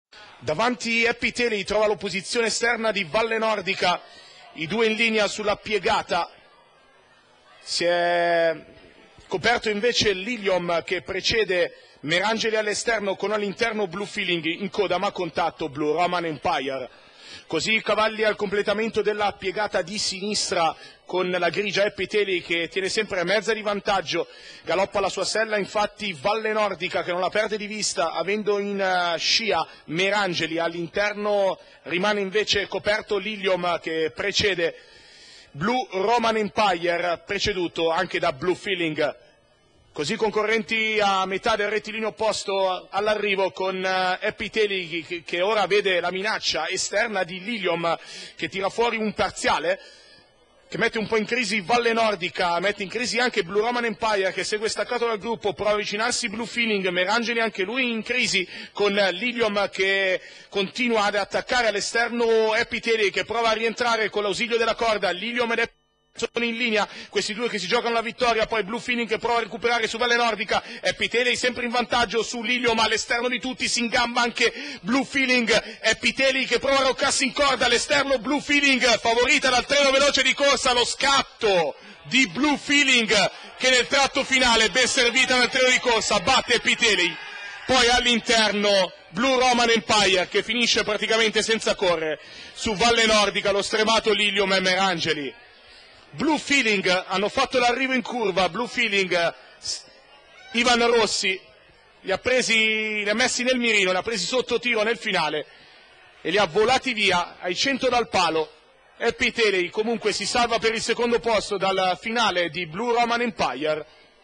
05/07/2014, Varese , Premio Taino
1. BLUE FEELING (ROSSI IVAN), 2. HAPPY TAILY (SANNA GAVINO), 3. BLU ROMAN EMPIRE (MARCIALIS JA.), 4. VALLE NORDICA (BOSSA FEDERICO), 5. LILIOM (MARCIALIS ANDREA), 6. MERANGELI (URRU S.)